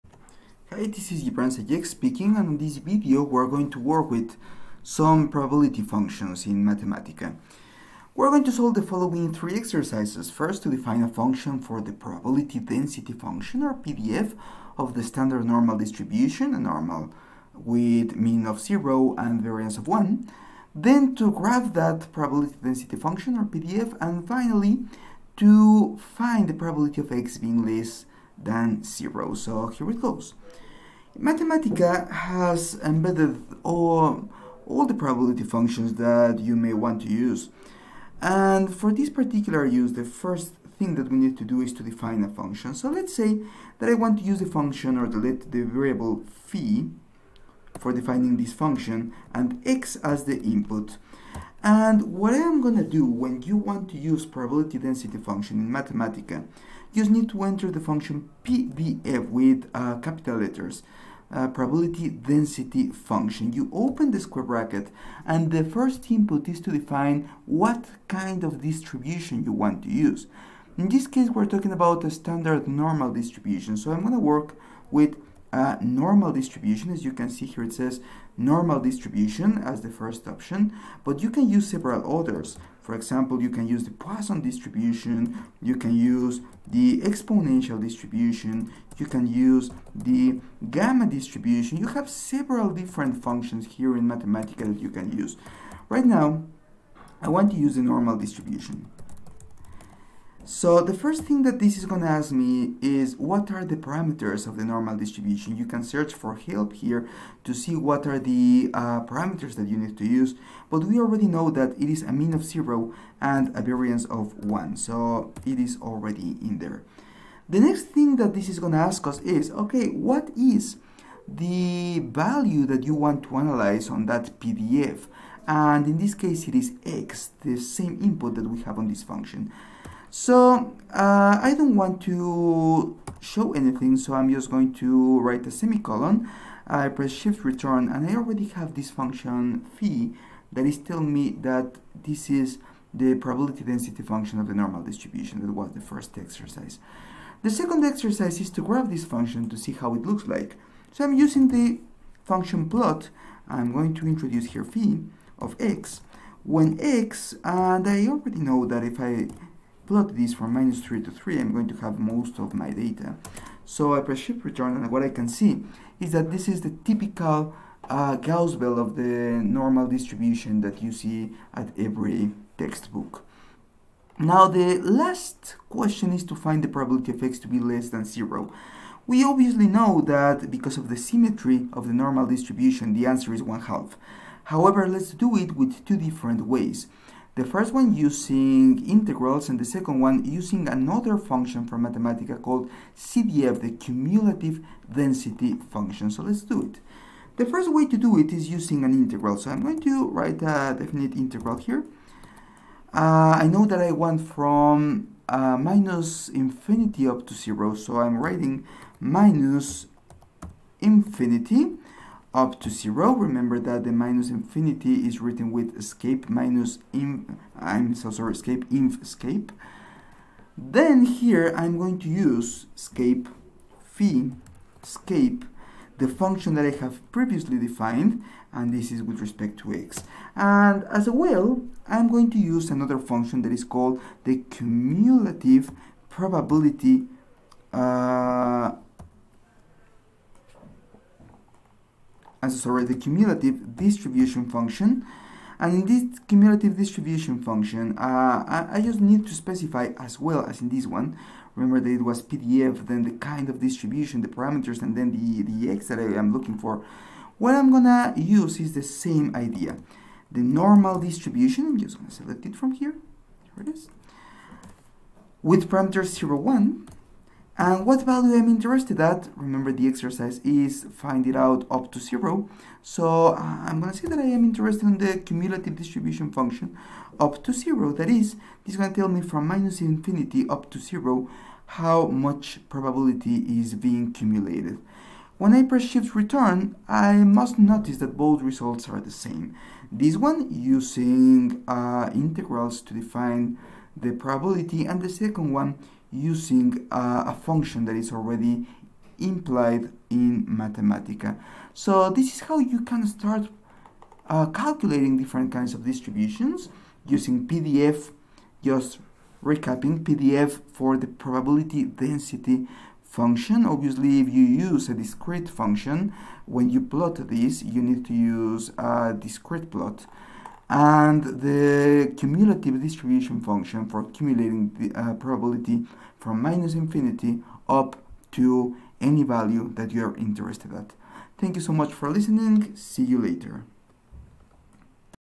PDF and CDF in Mathematica
In this video we explore the functions PDF (Probability Density Function) and CDF (Cumulative Distribution Function) for a standard normal distribution